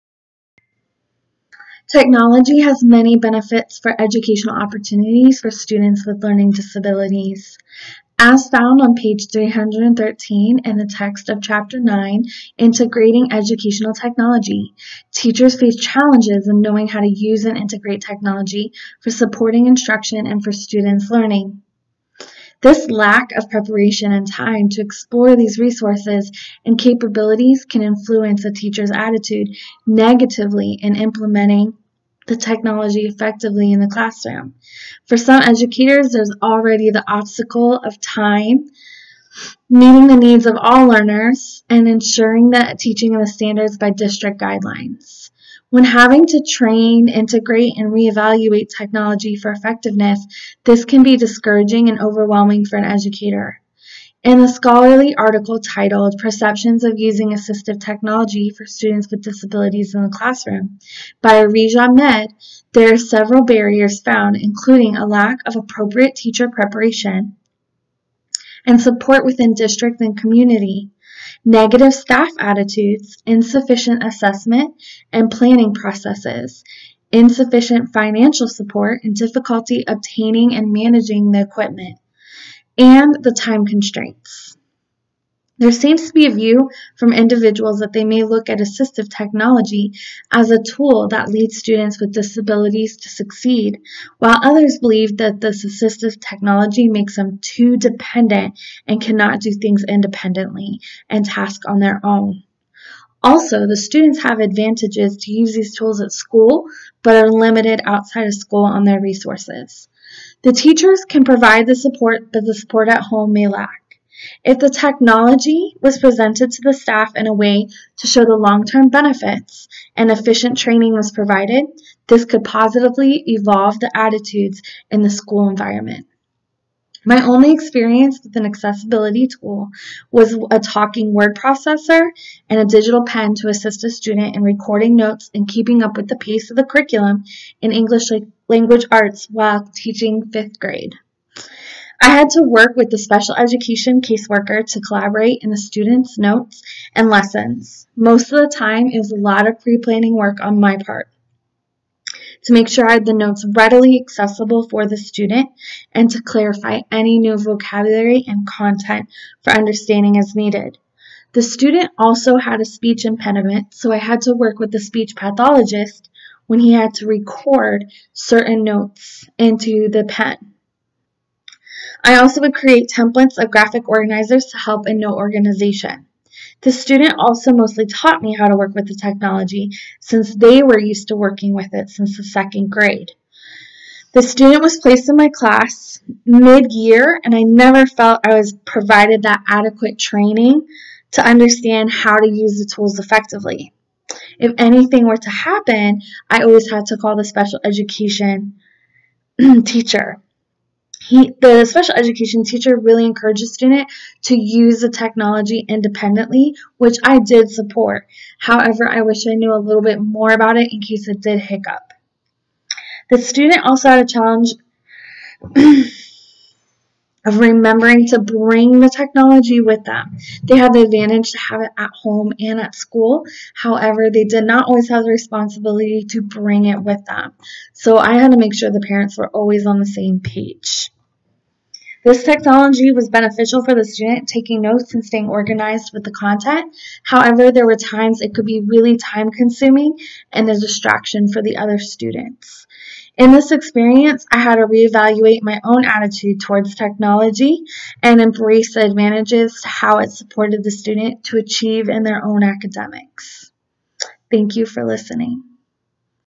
Video Blog 1- Accessibility Tool Integration
This video addresses the use of the technology to meet the needs in the classroom.